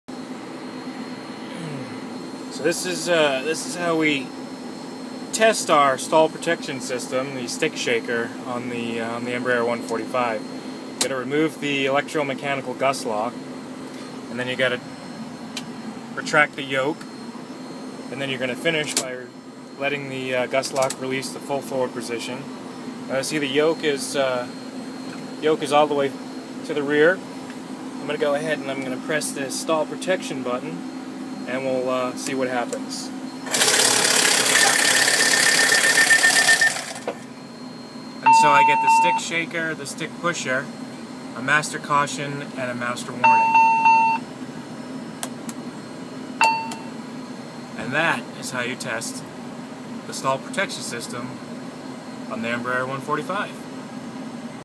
Embraer 145 Stall Protection Test
In this video you'll see how we test the stick shaker and stick pusher, components of the stall protection system, on this Embraer 145 regional jet.